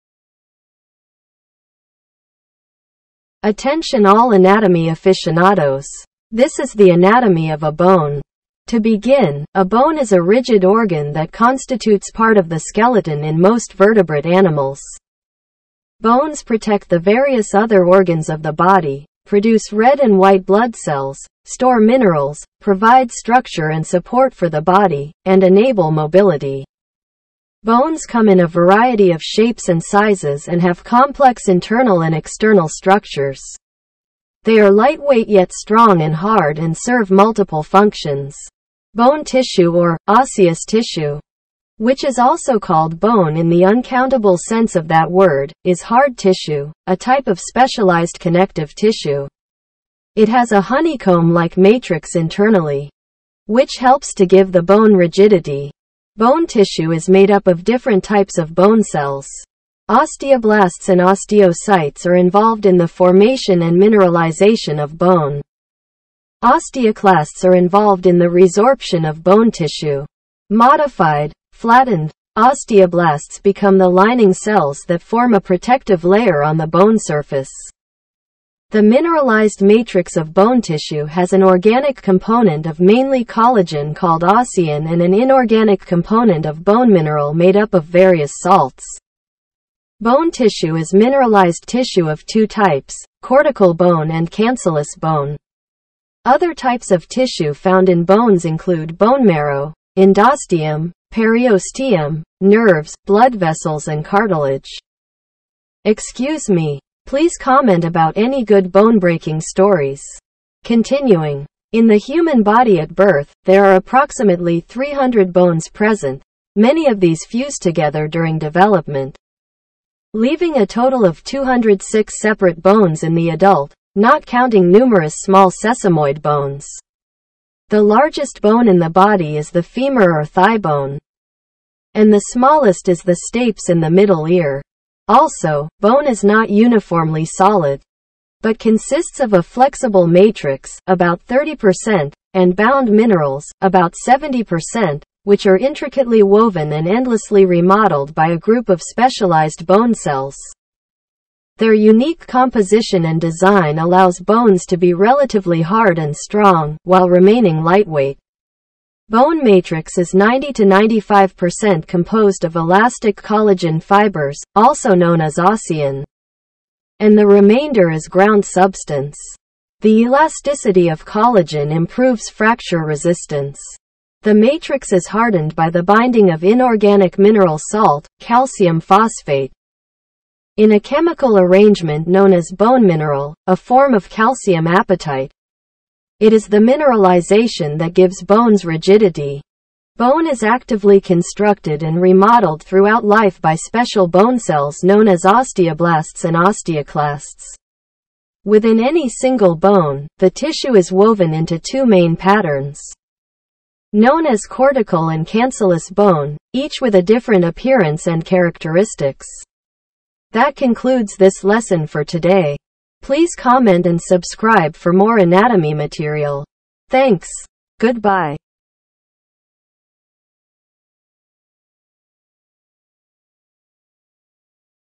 BONE ANATOMY
attention all anatomy aficionados. this is the anatomy of a bone. to begin, A bone is a rigid organ that constitutes part of the skeleton in most vertebrate animals. Bones protect the various other organs of the body, produce red and white blood cells, store minerals, provide structure and support for the body, and enable mobility. Bones come in a variety of shapes and sizes and have complex internal and external structures. They are lightweight yet strong and hard and serve multiple functions.

Bone tissue  or (osseous tissue), which is also called bone in the uncountable sense of that word, is hard tissue, a type of specialised connective tissue. It has a honeycomb-like matrix internally, which helps to give the bone rigidity. Bone tissue is made up of different types of bone cells. Osteoblasts and osteocytes are involved in the formation and mineralisation of bone; osteoclasts are involved in the resorption of bone tissue. Modified (flattened) osteoblasts become the lining cells that form a protective layer on the bone surface. The mineralised matrix of bone tissue has an organic component of mainly collagen called ossein and an inorganic component of bone mineral made up of various salts. Bone tissue is mineralized tissue of two types, cortical bone and cancellous bone. Other types of tissue found in bones include bone marrow, endosteum, periosteum, nerves, blood vessels and cartilage. excuse me. please comment about any good bone breaking stories. continuing.

In the human body at birth, there are approximately 300 bones present; many of these fuse together during development, leaving a total of 206 separate bones in the adult, not counting numerous small sesamoid bones. The largest bone in the body is the femur or thigh-bone, and the smallest is the stapes in the middle ear.
    also,  Bone is not uniformly solid, but consists of a flexible matrix (about 30%) and bound minerals (about 70%) which are intricately woven and endlessly remodeled by a group of specialized bone cells. Their unique composition and design allows bones to be relatively hard and strong, while remaining lightweight.

Bone matrix is 90 to 95% composed of elastic collagen fibers, also known as ossein, and the remainder is ground substance. The elasticity of collagen improves fracture resistance. The matrix is hardened by the binding of inorganic mineral salt, calcium phosphate, in a chemical arrangement known as bone mineral, a form of calcium apatite.  It is the mineralization that gives bones rigidity.

Bone is actively constructed and remodeled throughout life by special bone cells known as osteoblasts and osteoclasts. Within any single bone, the tissue is woven into two main patterns, known as cortical and cancellous bone, each with a different appearance and characteristics.